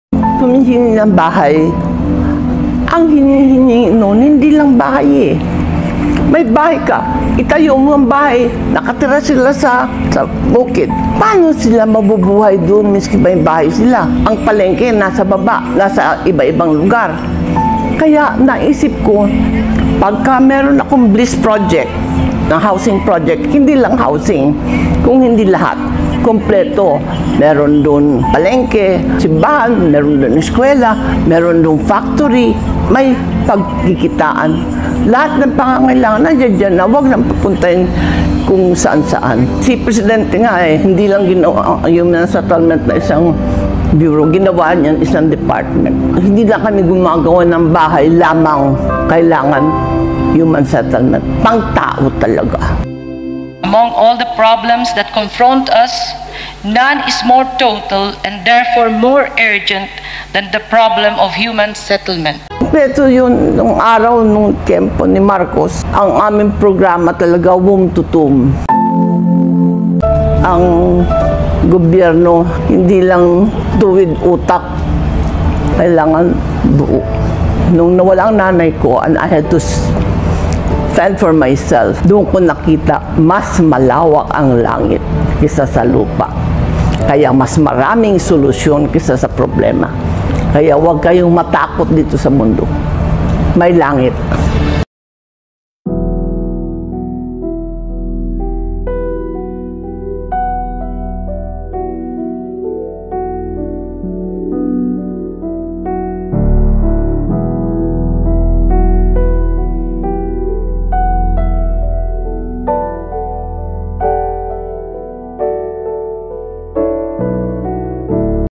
PBBM || ITO ANG TUNAY NA NAGMAMAHAL AT MAY MALASAKIT SA BAYAN MARCOS FAMILY
napakahusay at mabuti ang pagpapalaki ng mga magulang kay PBBM..MAAM MELDY AT F.MARCOS SALAMAT BINIGYAN NYO KAMI NG ISANG PINUNO NA MAGANDA ANG PAGPAPALAKI SA KANYA..